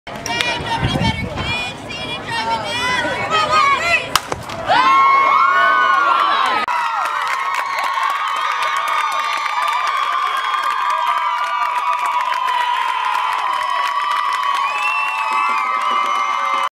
Brandi Giles home run @ La Porte 2013
North Shore Mustang junior Brandi Giles hitting home run at La Porte High School in 2013.